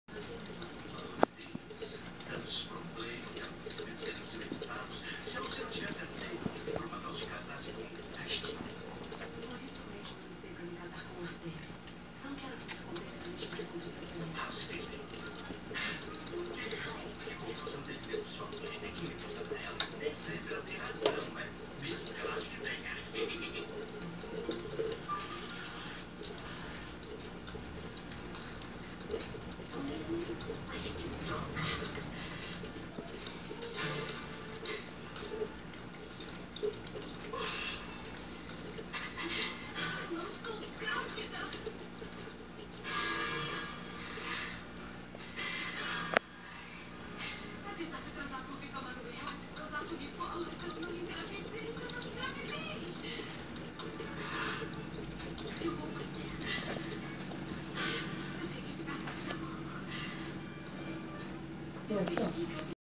Shih tzu bebedouro de bilha
2 meses, primeiro dia em casa e já sabe beber direitinho no bebedouro de bilha